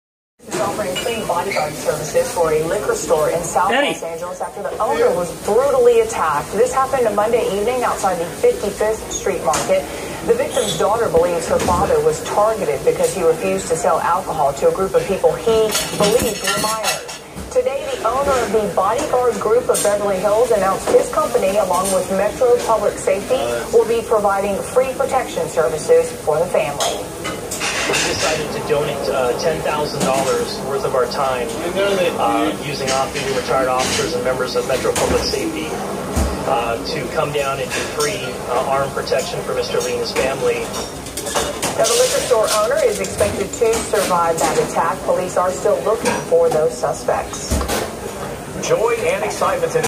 NEWS VIDEO: Kris Herzog FREE Bodyguards for crime victim, The Bodyguard Group of Beverly Hills 90210
Owner Kris Herzog gives FREE Bodyguard services for Violent crime victim in South Los Angeles at the 55th street Market Mr. Lee Owner , 5428 S. Hooper Ave, Los Angeles, CA. The Bodyguard Group of Beverly Hills 90210, Los Angeles bodyguard for hire, @007bodyguards , @KrisHerzog007